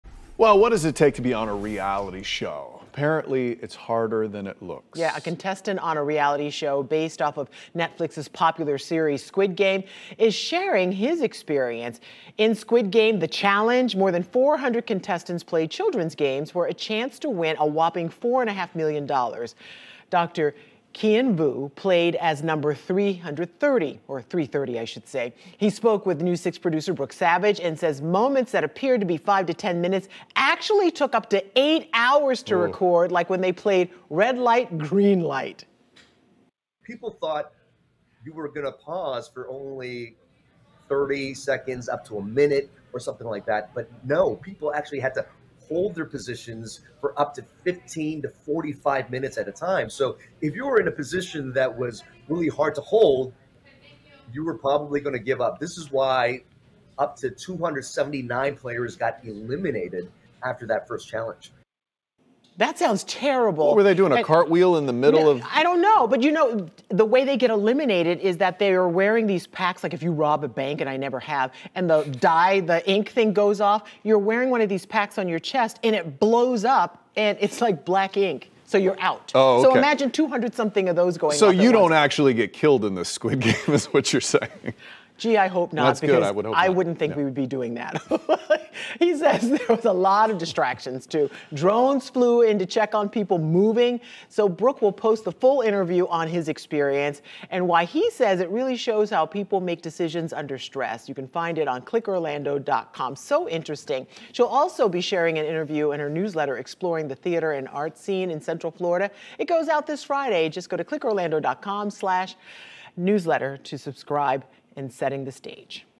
Reality contestant shares behind-the-scenes experience of Squid Game: The Challenge with News 6
What does it take to be on a reality TV show? There’s a lot more that goes into it than one might think, especially a show based off another show, where players compete until their death.